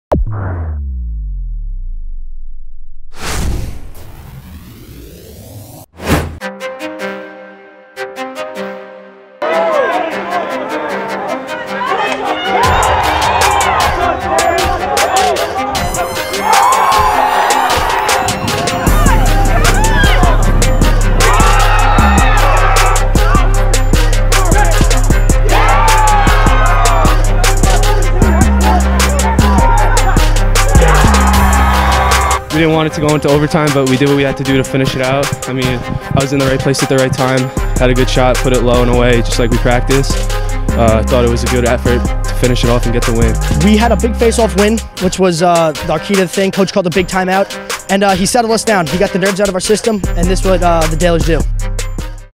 Farmingdale Beats Lynbrook 9-8 in OT
Farmingdale Boys Lacrosse wins a thriller 9-8 in overtime over a tough Lynbrook team on March 20, 2019. Jack Cavaioli with the game winner for the Dalers.